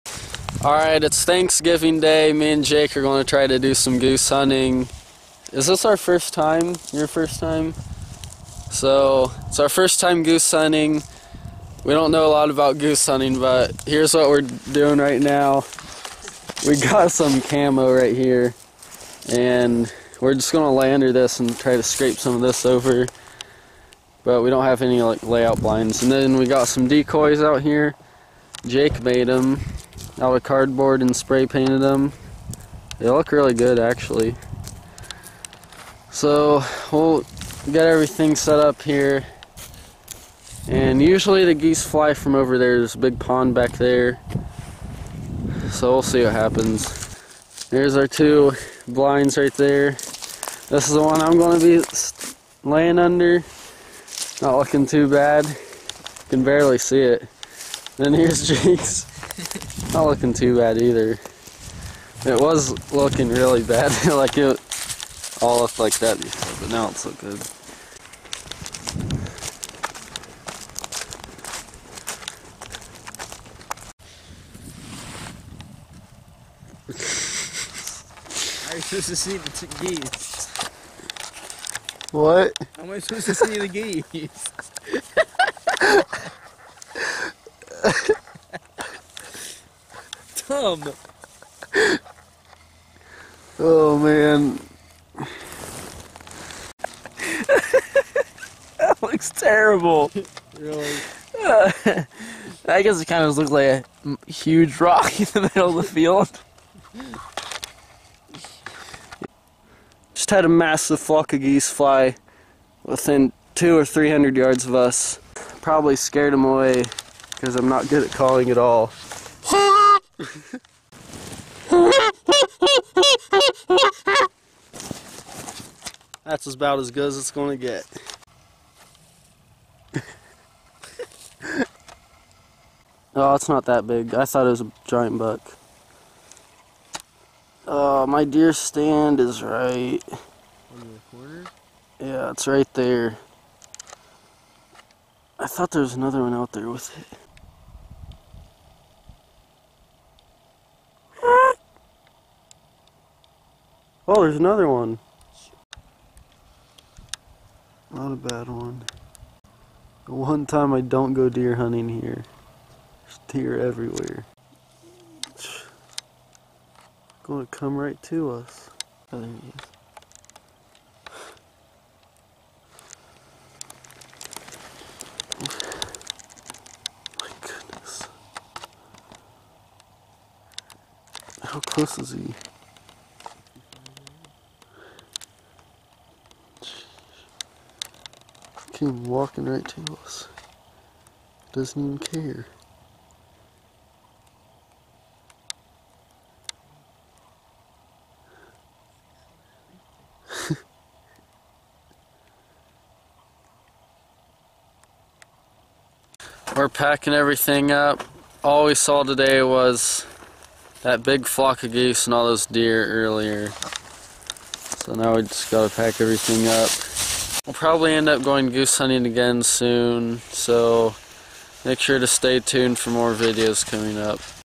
We Went Goose Hunting but Called in a Deer!
This was are first time ever calling in geese to shoot. Even though our hunt was unsuccessful we still had a great time. We have killed a couple geese before but that was just from running out into a flock of them and unloading our shotguns on the flock of geese.